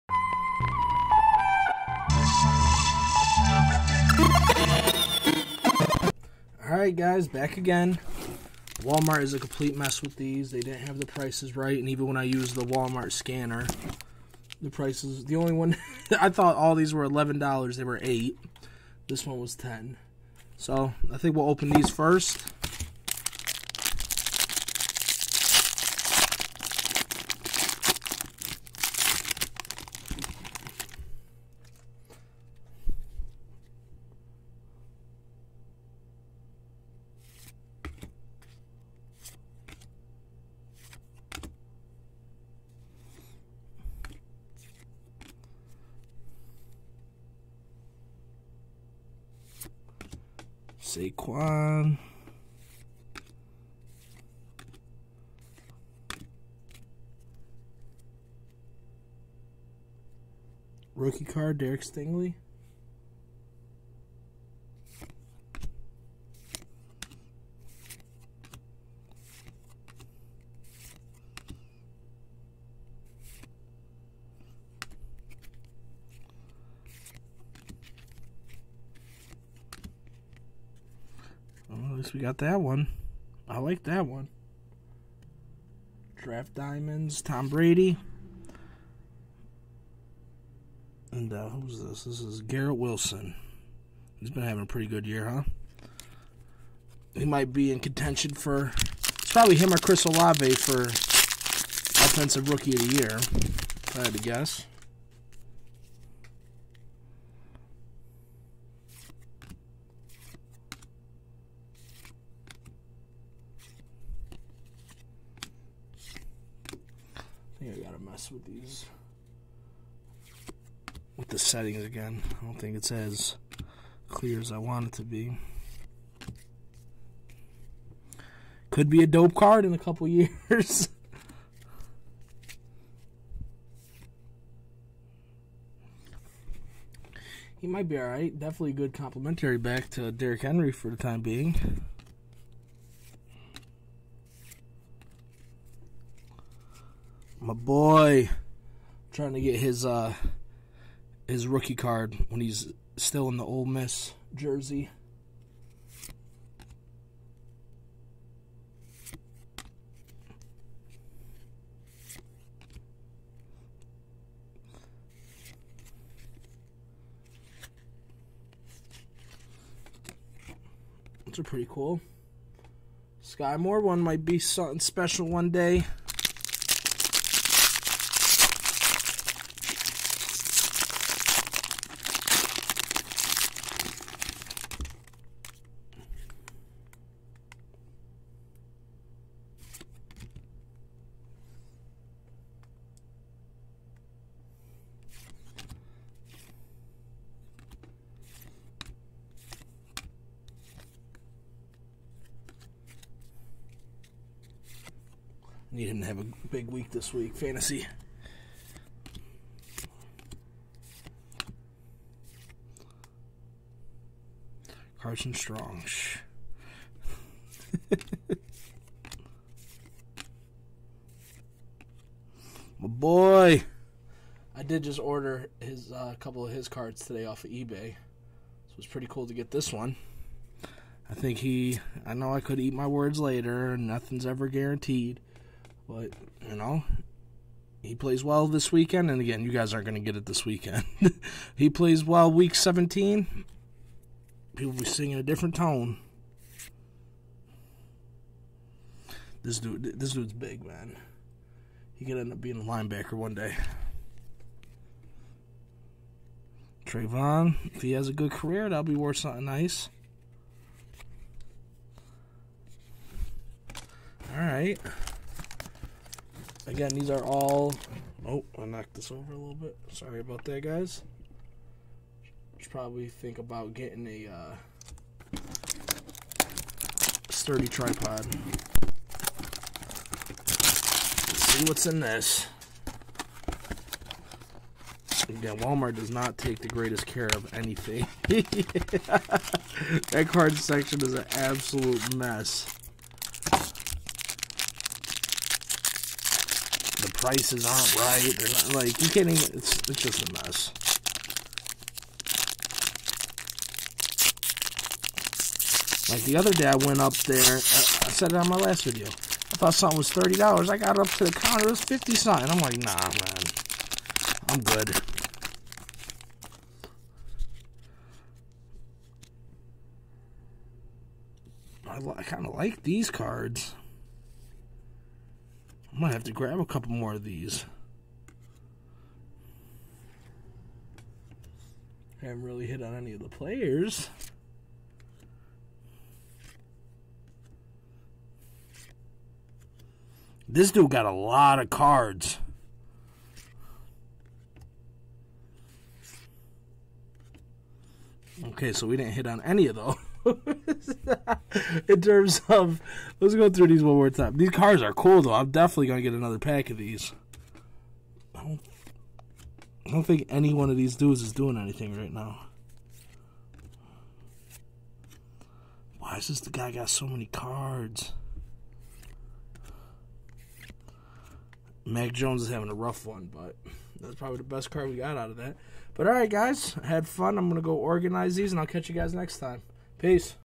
Breaking OPEN SOME NFL 22' Absolute and Wild Card packs
Let's open some packs of NFL cards!

Opening:
NFL 22' Absolute and Wild Card packs

How do you start again?
Where should you buy?
How much will it cost?
Will I ever catch up?

Let's learn and have some fun opening packs!